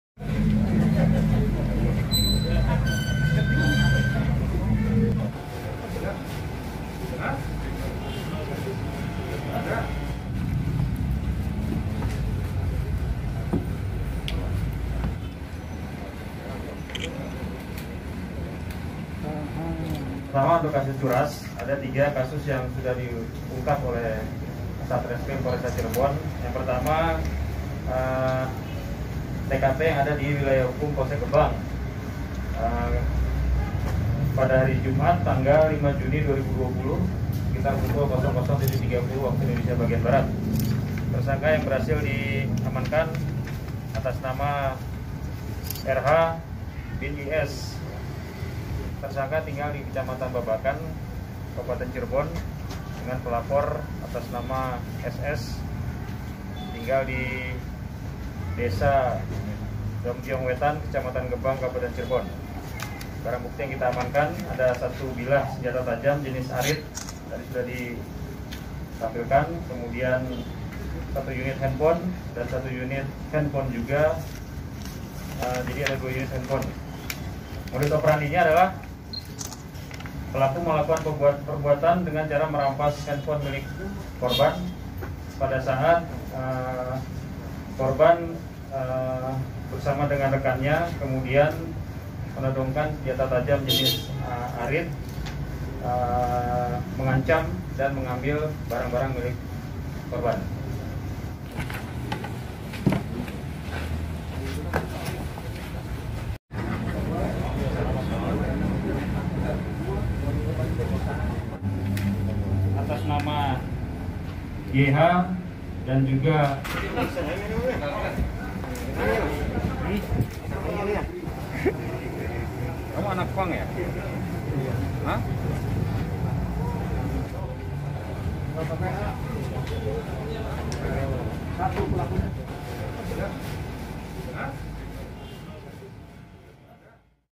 Buruh di Cirebon Todongkan Arit kemudian Rampas Ponsel, Kini Ditangkap Polisi
Laporan Wartawan Tribuncirebon.com, Ahmad Imam Baehaqi

TRIBUNCIREBON.COM, CIREBON - Jajaran Satuan Reskrim Polresta Cirebon menangkap buruh berinisial RH (23).

Warga Kecamatan Babakan, Kabupaten Cirebon, itu dibekuk karena merampas ponsel milik pemuda bernama Sis Suryaman (18).

Kapolresta Cirebon, Kombes Pol M Syahduddi, didampingi Kasat Reskrim, Kompol Rina Perwitasari, mengatakan, peristiwa itu terjadi pada Jumat (5/6/2020) kira-kira pukul 00.30 WIB.

Kala itu, korban bersama rekannya tengah nongkrong di pinggir jalan kawasan Desa Dompyong Wetan, Kecamatan Gebang, Kabupaten Cirebon.

"Tiba-tiba mereka didatangi pelaku yang membawa arit," ujar M Syahduddi saat konferensi pers di Mapolresta Cirebon, Jalan R Dewi Sartika, Kecamatan Sumber, Kabupaten Cirebon, Senin (6/7/2020).

Ia mengatakan, pelaku mengancam korban sambil mengacungkan arit yang panjangnya kira-kira 60 cm tersebut.

Selanjutnya RH meminta korban untuk menyerahkan barang-barang berharga miliknya.

Korban yang tidak berdaya pun terpaksa menyerahkan ponselnya kepada pelaku.

Namun, saat pelaku hendak kabur korban dan teman-temannya berusaha mencegahnya.

Mereka mencekik dan memiting pelaku yang kala itu hendak kabur mengendarai sepeda motor.

"Pelaku langsung mengayunkan arit yang dibawanya ke arah korban, dan mengenai bahu kanan korban," kata M Syahduddi.

Beruntung, saat itu pelaku kondisinya dipiting sehingga bacokan aritnya hanya mengakibatkan korban mengalami luka lecet.

Korban dan teman-temannya pun mencari pertolongan warga dan melapor ke Polsek Gebang.

Menerima laporan tersebut, petugas piket Polsek Gebang pun langsung mendatangi lokasi kejadian dan berhasil mengamankan RH.

"Barang bukti yang diamankan di antaranya, dua ponsel dan satu bilah arit," ujar M Syahduddi.